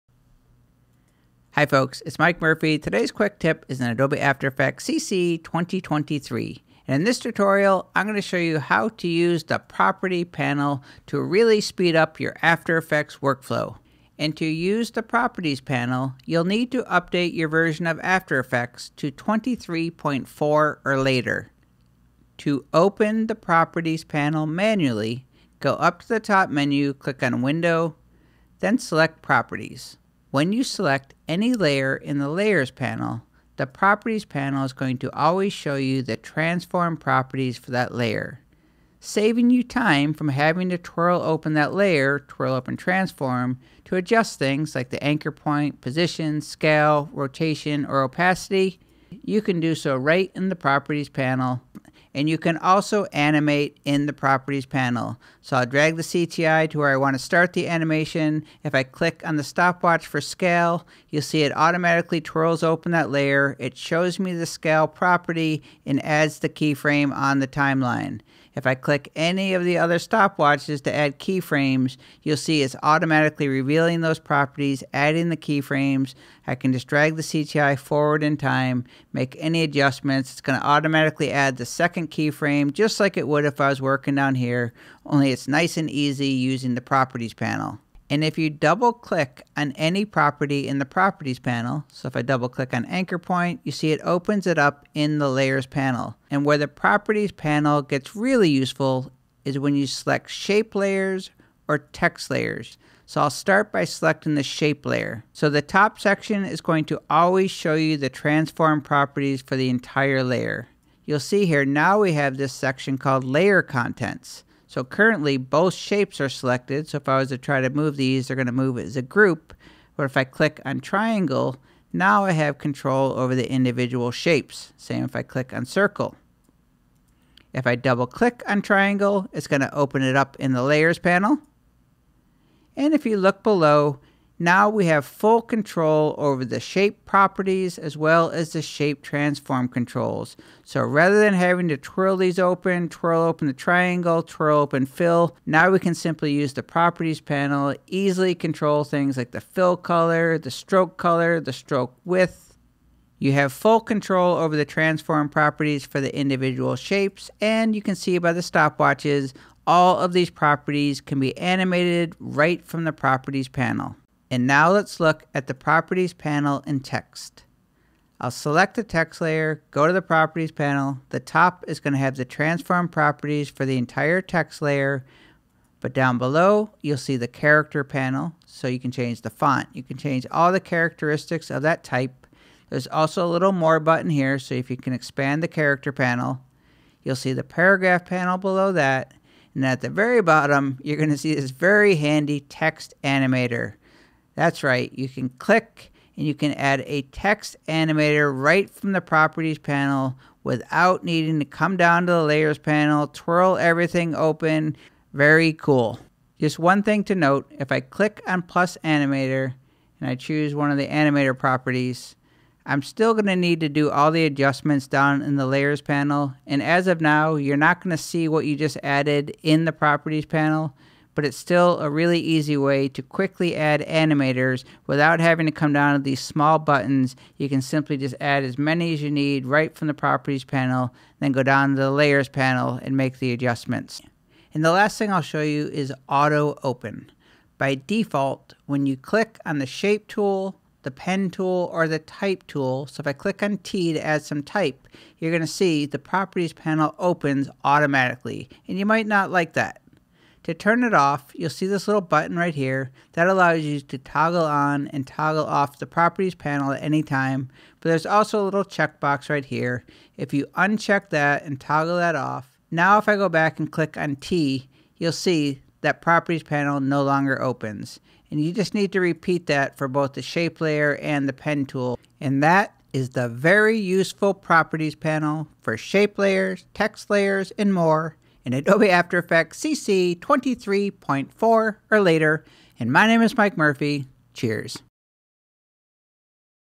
Properties Panel in After Effects
After Effects CC 2023: How To Use Properties Panel

This tutorial will show you how to use the new Properties Panel in Adobe After Effects CC 2023 to speed up making animations by cutting down on the ’twirl time’ of opening layers to access properties and settings.

After Effects Version Required:23.4.0 or later

The Properties Panel:
1. Will speed up workflow and make it easier and faster to animate layers
2. Lives next to Composition Window
3. Add animation to your text, shapes, and layers without changing the timeline view.
4. Access to the important properties and controls for your selected layer or layers

Chapters:00:00  Intro: About This Tutorial
00:21  How To Open Properties Panel (Window Menu)
00:38  Properties Panel: Transform Properties
00:56  Adding Keyframes from Properties Panel
01:27   Double-Click On Properties
01:45  Selecting Shape Layers
02:17   Shape Properties & Transform Controls
02:44  Properties Panel: Text Layers
03:13  Text Animation: Add Animators
03:53  Auto-Open Settings
04:13  Auto-Open Settings

How To Open Properties Panel (Manual):
1. Go to the top Menu
2. Click on ‘Window’
3. Select ‘Properties’

How To Use Properties Panel:
1. Select any layer
2. Look in Properties Panel
3. The Top section controls Layer Transform controls
4. Adjust Transform Properties
5. Animate by click on stopwatch to add a keyframe
6. Move CTT (play-head) forward in time and make an adjustment to property
7. 2nd keyframe added automatically
8. Drag CTI to beginning of timeline and tap Spacebar to play

Tip: Double Click on any property to open in Layers Panel

Text & Shape Layers:
1. Select a Shape or Text Layer
2. Go to Properties Panel
3. Use top Layer Controls to change Layer Transforms
4. Use Shape & Text Controls to change fills, strokes, fonts, and more
5. Animate any property in the Properties Panel
6. For Text, add Text Animators at the bottom of the Properties Pane

Auto-Open & Toggle On/Off Properties Panel (Shape, Pen, Text Tools)
1. Go to the top Toolbar
2. Select Shape Tool
3. Click icon to Toggle On/Off Properties Panel
4. Uncheck to stop Properties Panel from Auto-opening
5. Repeat to Pen Tool & Text Tool
6. To open Properties Panel again, go to Window…Properties